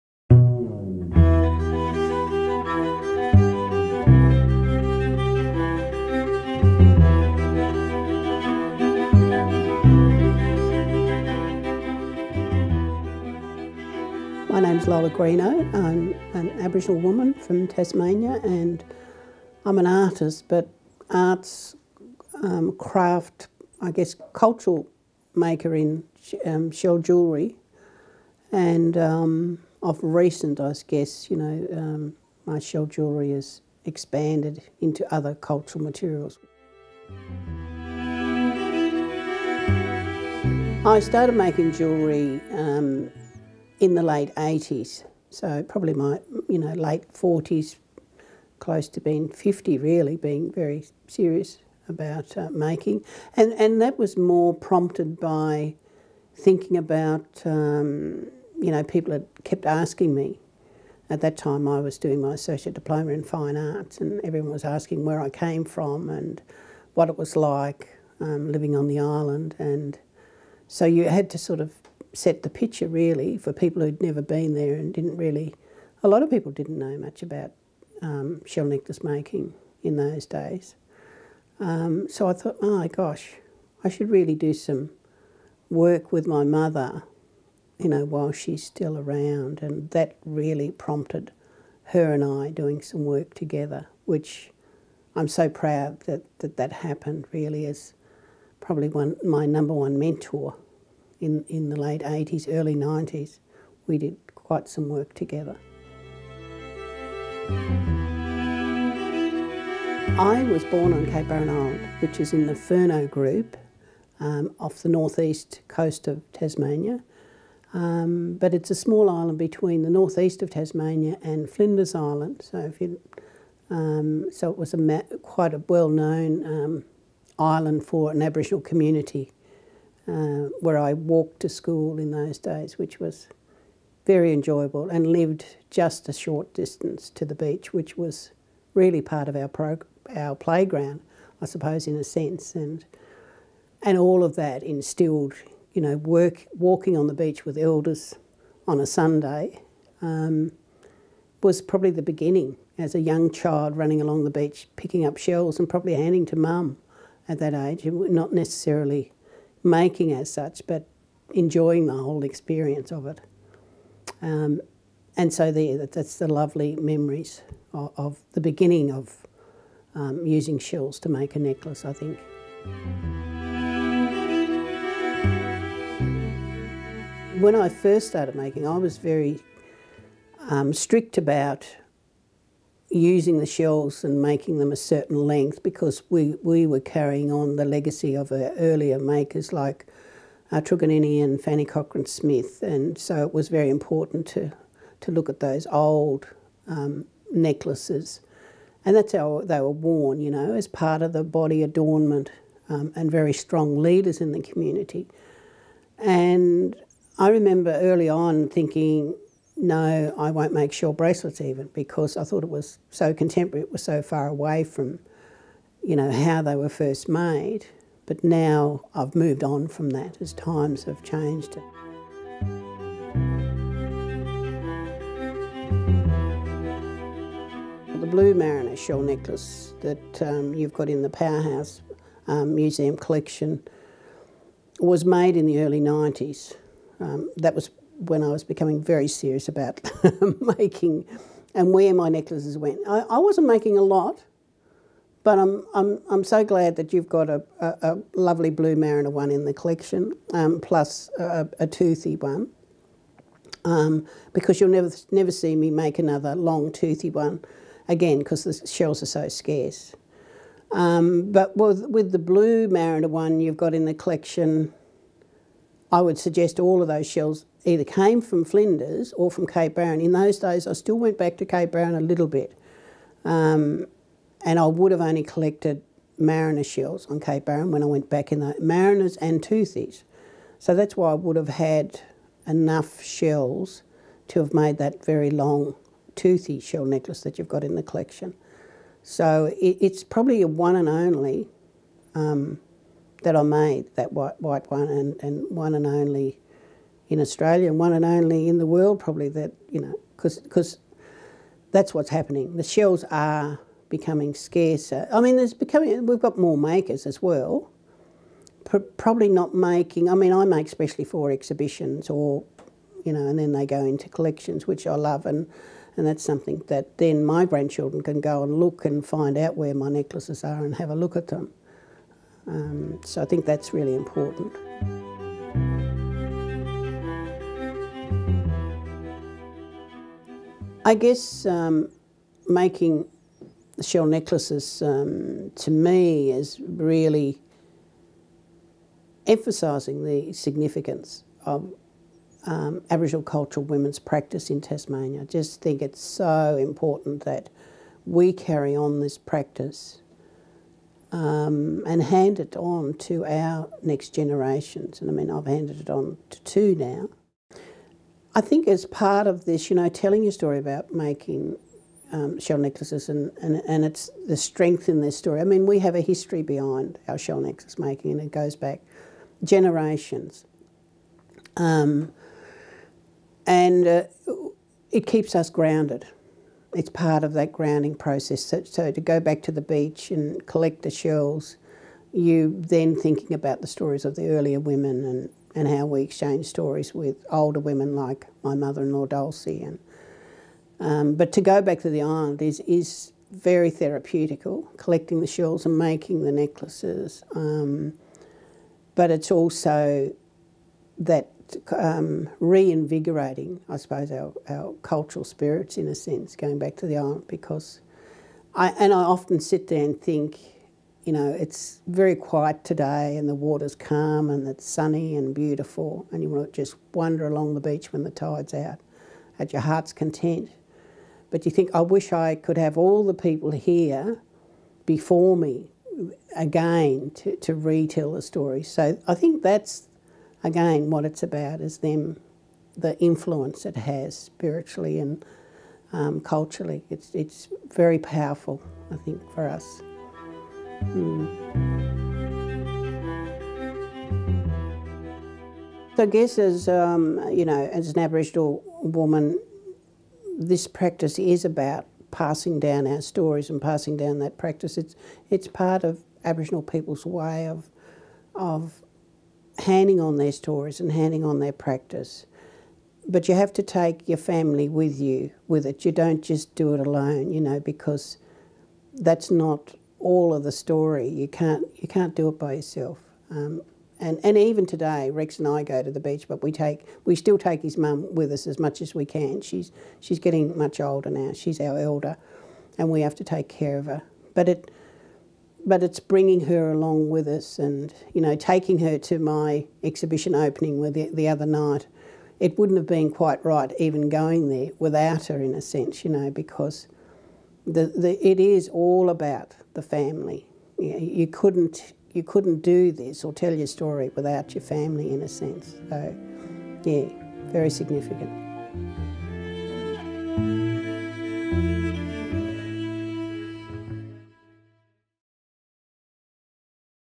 Lola Greeno Interview
Interview with Lola Greeno for A Fine Possession exhibition at the Powerhouse Museum, Sydney.

Production by Leonie Jones

Still photography by Marinco Kojdanovski & Rex Greeno